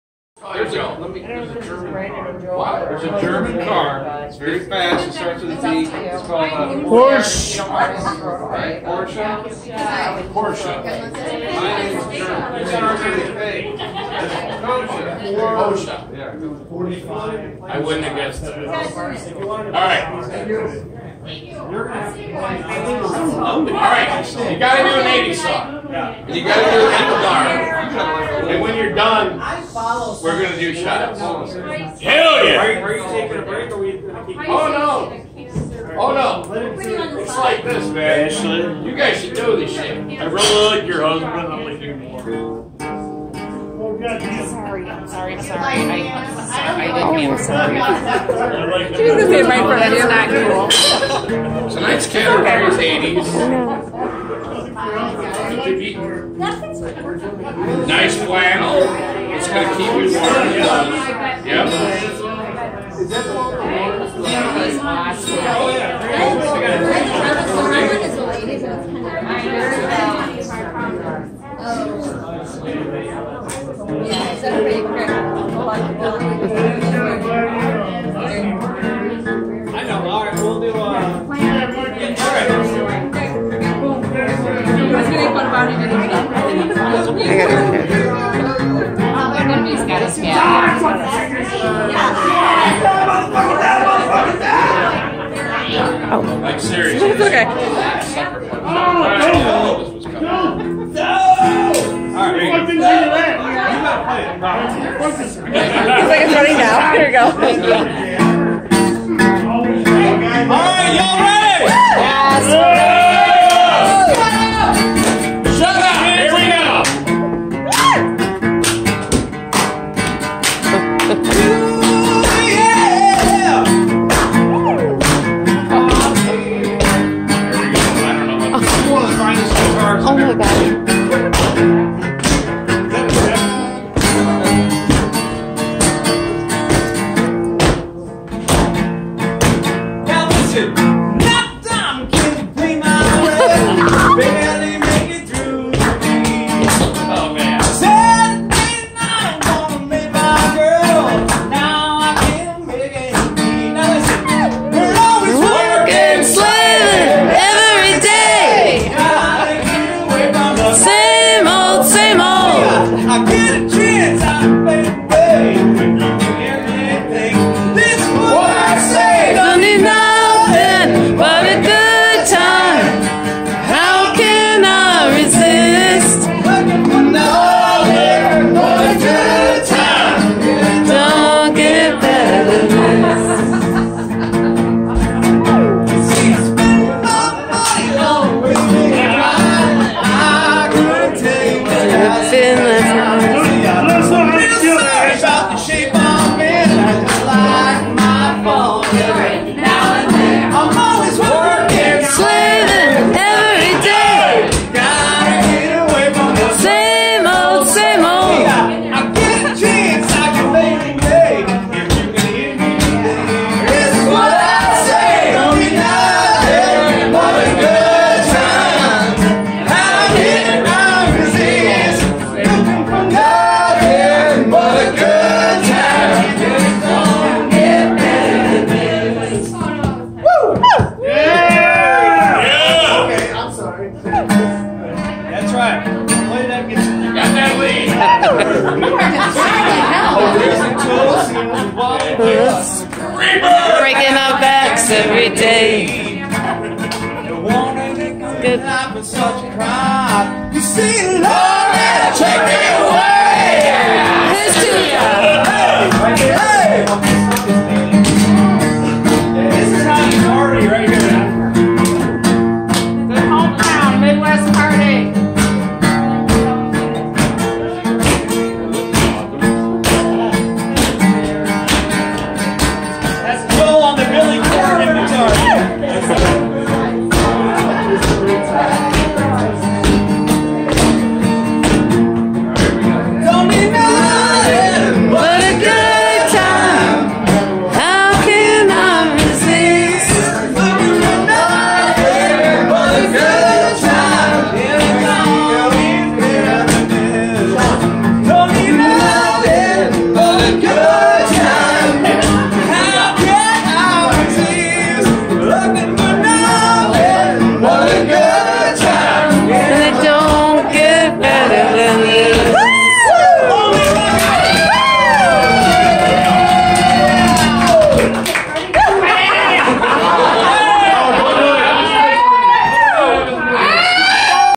Brandon Gibbs & Joel Kosche after party
Good times at the Top Fuel Saloon on Braidwood, Illinois